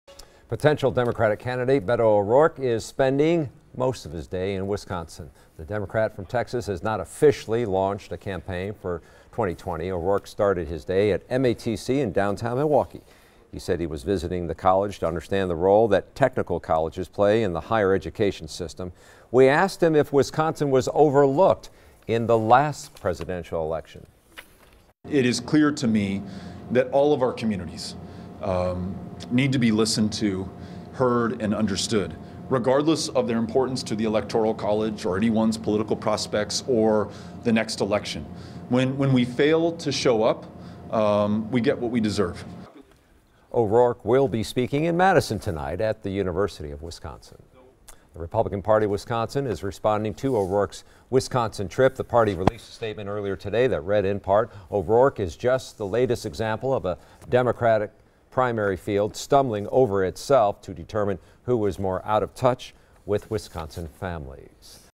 Potential 2020 candidate Beto O'Rourke planning two stops in Wisconsin Friday
Potential 2020 Democratic presidential candidate Beto O'Rourke is coming to the University of Wisconsin-Madison campus Friday.